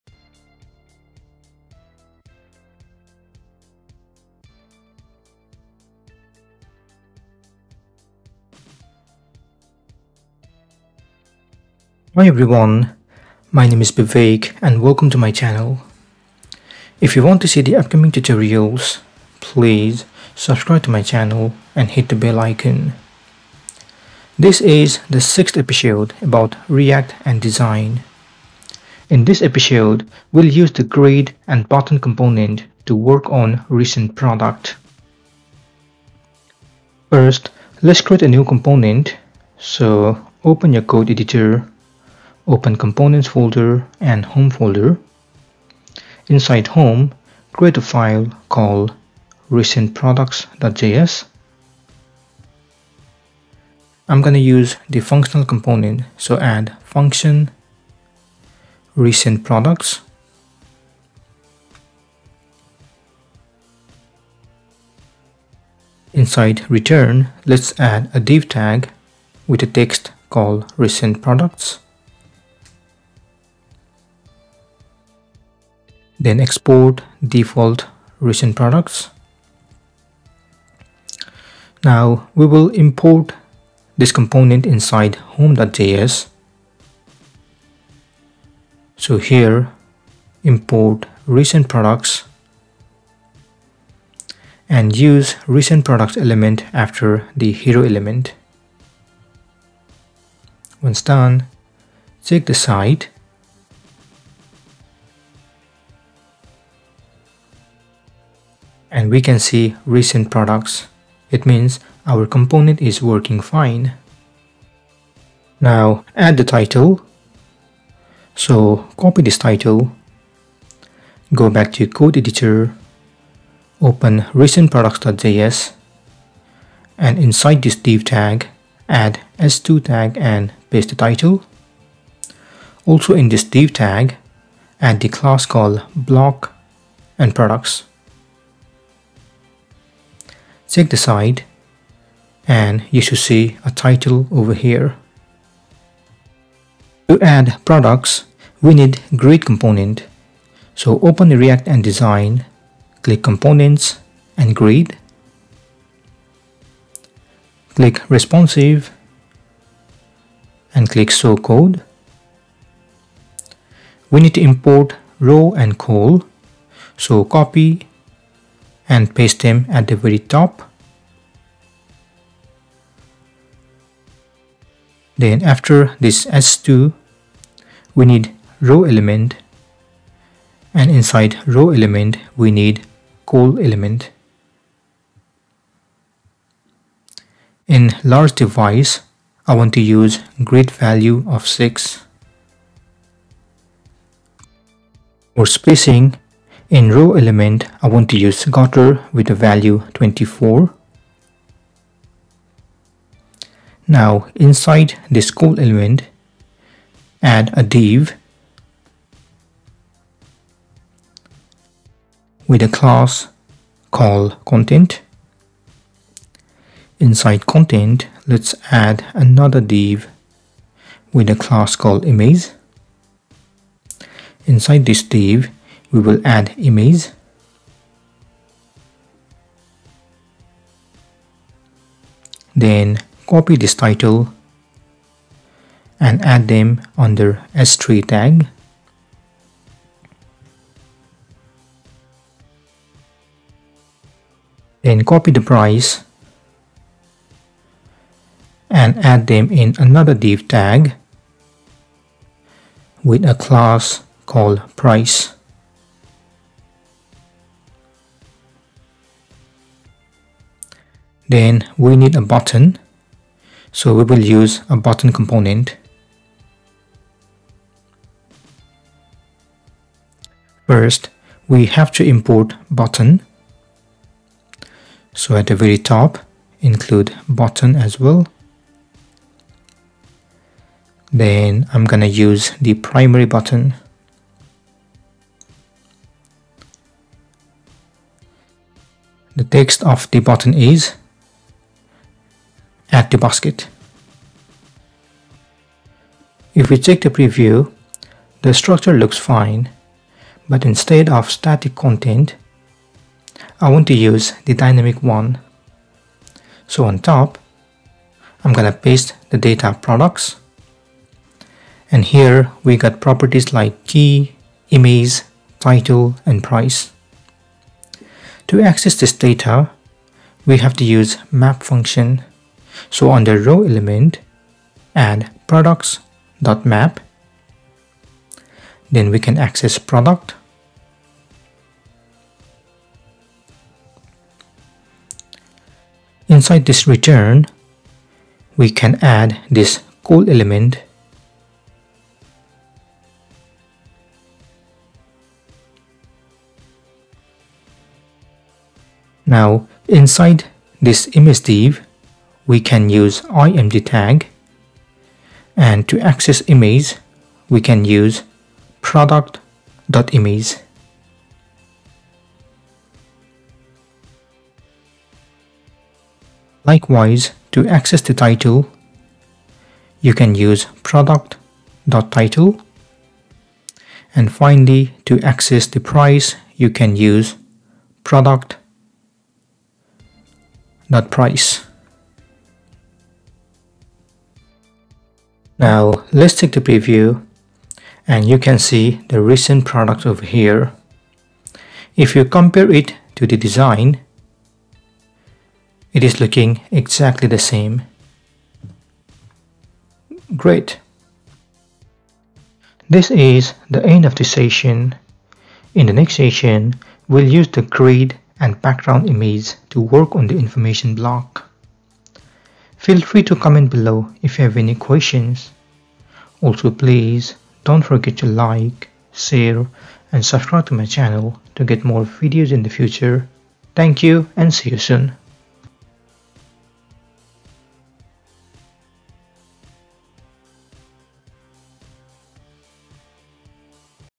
React JS #6 - Use grid and button component to work on recent products
Introduction:
Learn React Ant Design with Real Project.
In this tutorial, I'll show you how to use grid and button component to work on recent products.